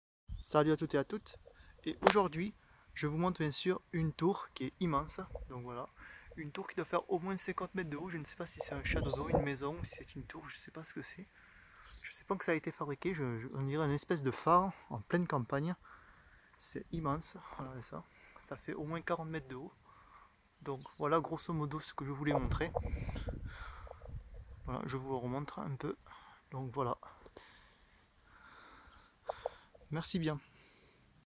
Tour géant de Puylagarde dans le Tarn et Garonne
Elle mesure 30 a 40 mètres de haut
Elle fut construit au 14eme siècle

merci de me suivre et de regarder tous ce que je fais pour ma chaîne